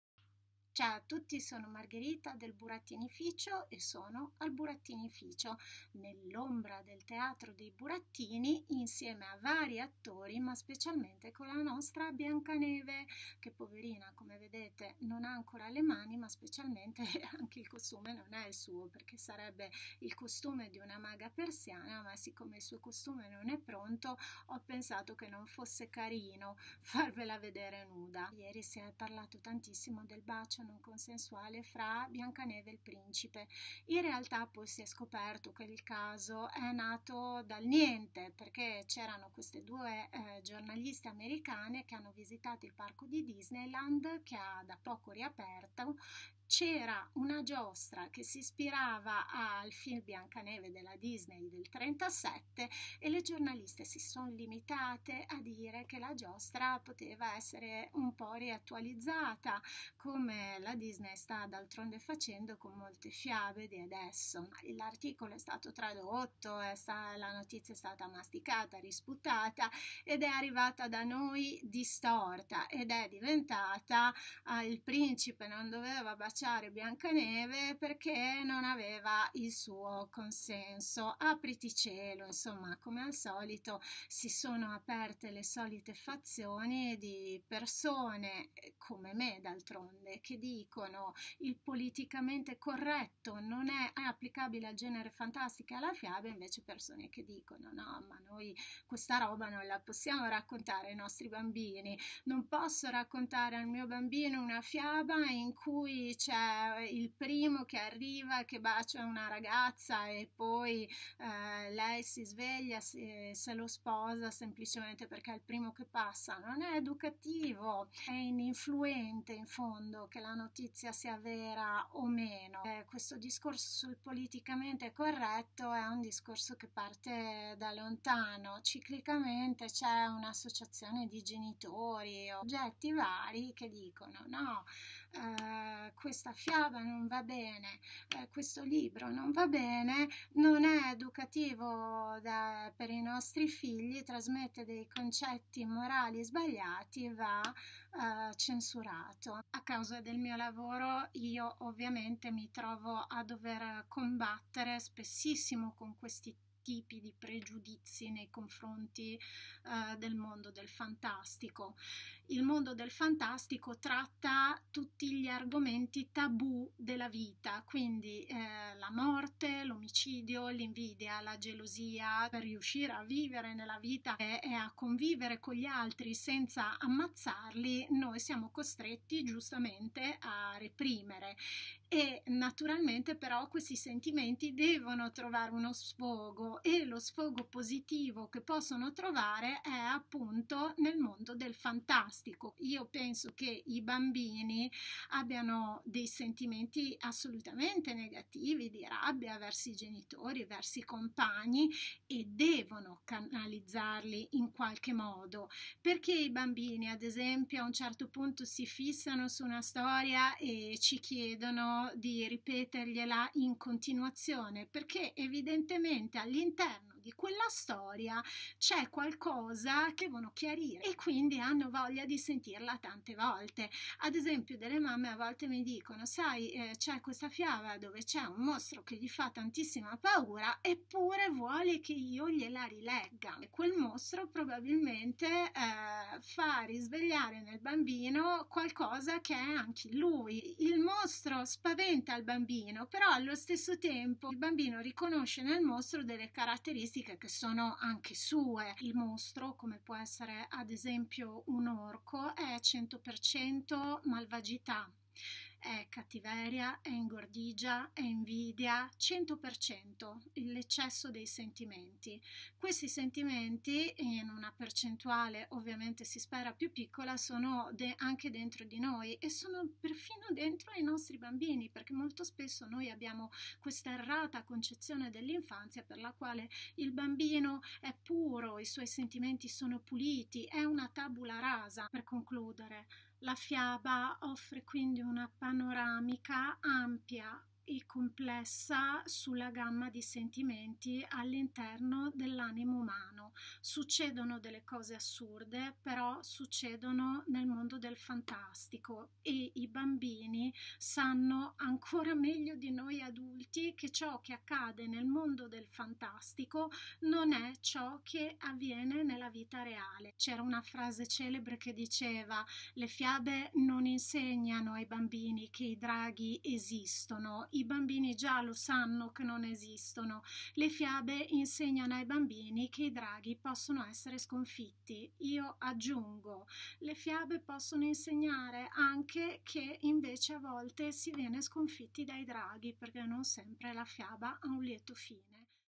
Biancaneve e il bacio non consensuale
Politicamente corretto e fiaba. Raccontando le fiabe insegniamo valori sbagliati ai nostri figli?